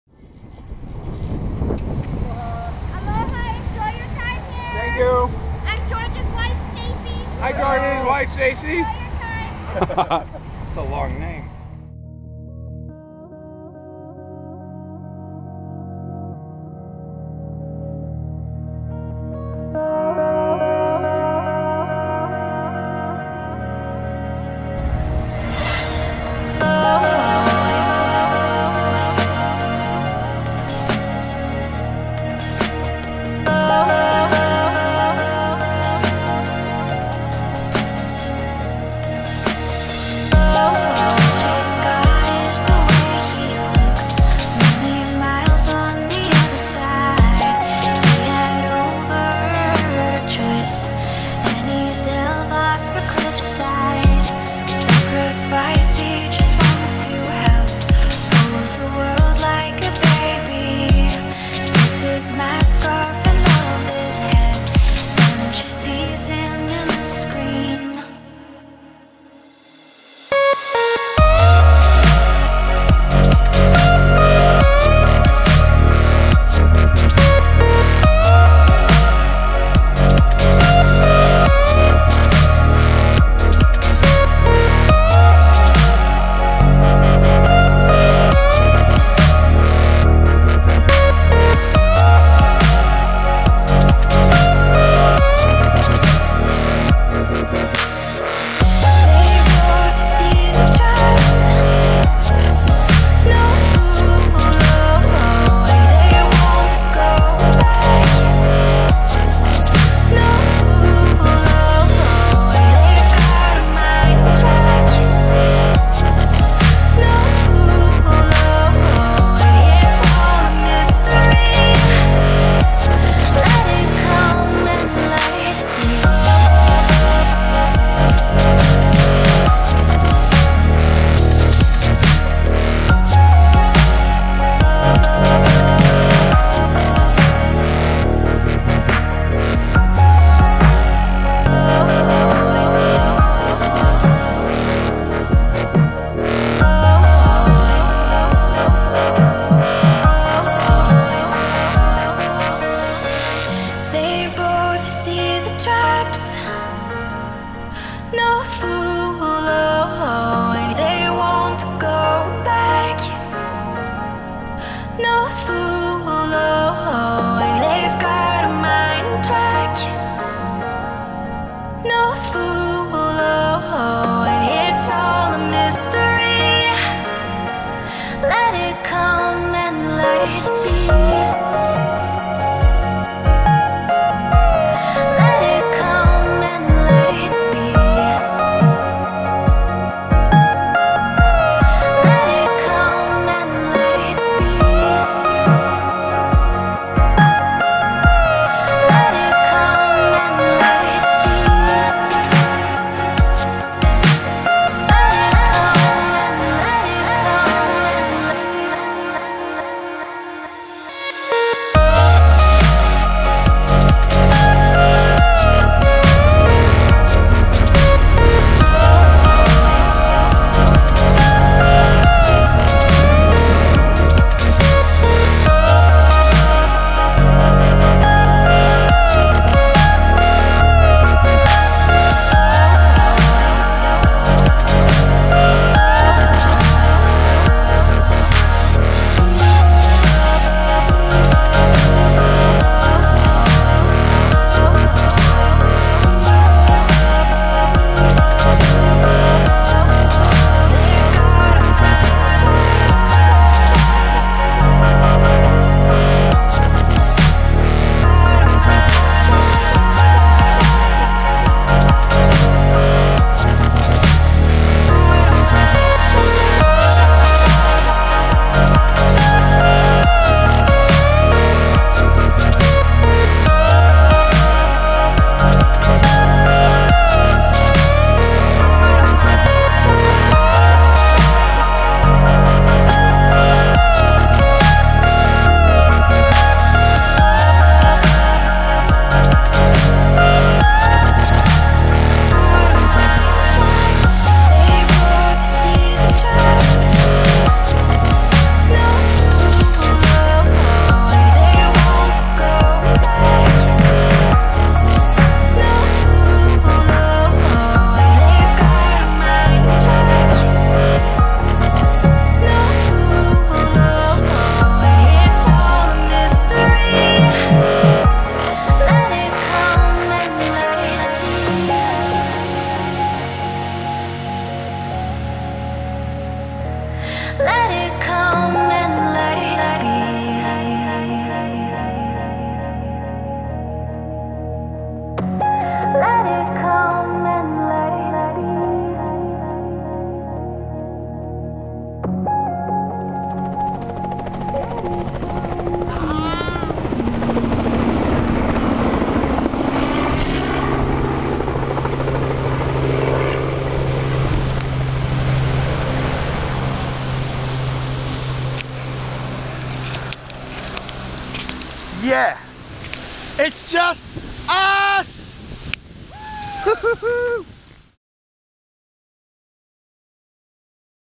FPV - paradise - Hawaii
I still can't believe I got to experience the most epic FPV trip ever. Huge thanks to Rotorsports, Lumenier and ImmersionRC (Scot, Tim and Tony) for making this incredible FPV tour of Hawaii a reality. Mahalo to all our Hawaiian friends that made us feel so welcome with your generosity and smiles. And to the amazing pilots that I got to hang out and fly with, I miss all of you.

I hope very much to fly with you all again some day.

Shots at 0:54 to 1:10 and 3:01 where flown with my Lumenier QAV400G using Lumenier 30A ESCs, Tiger 1100kv motors, 8inch slowfly props, Naze, EZUHF, fatshark 250mW vtx, immersionRC spironet antenna, Lumenier 3300mah 4s lipo.